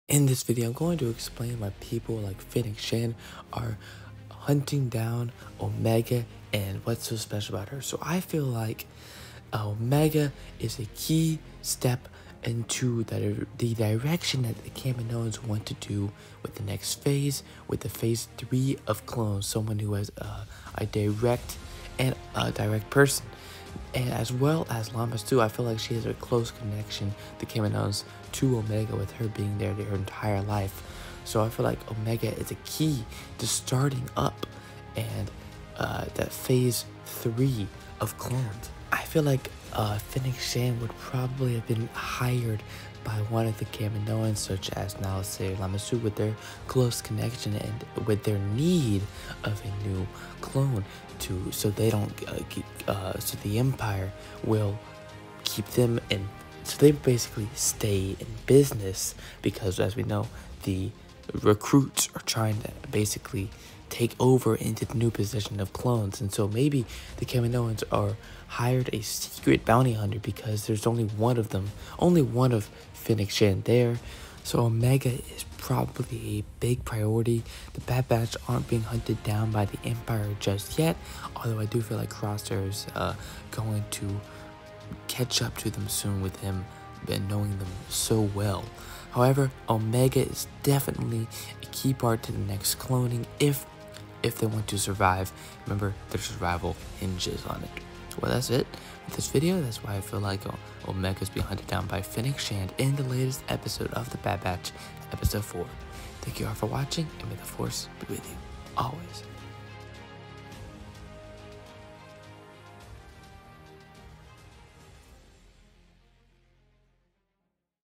Why Omega Is Being Hunted Down - Bad Batch Episode 4
is it because of the kaminoans

VIDEOS YOU WILL LIKE